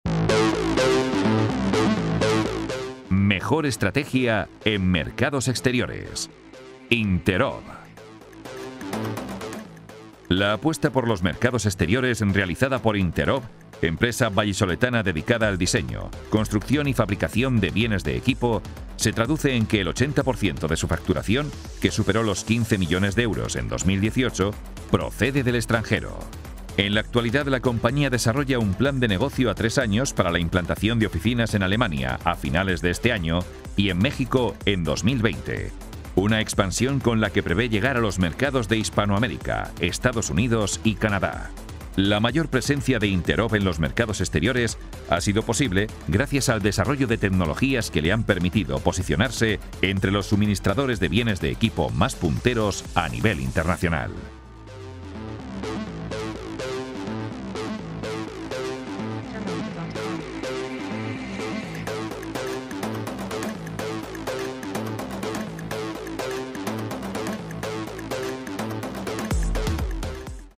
Premio a la Mejor Estrategia en Mercados Exteriores: Interob
La apuesta por los mercados exteriores realizada por Interob, empresa vallisoletana dedicada al diseño, construcción y fabricación de bienes de equipo, se traduce en que el 80% de su facturación, que superó los 15 millones de euros en 2018, procede del extranjero. En la actualidad, la compañía desarrolla un plan de negocio a tres años para la implantación de oficinas en Alemania a finales de este año y en México en 2020, una expansión con la que prevé llegar a los mercados de Hispanoamérica, EE UU y Canadá. La mayor presencia de Interob en los mercados exteriores ha sido posible gracias al desarrollo de tecnologías que le han permitido posicionarse entre los suministrados de bienes de equipo más punteros a nivel internacional.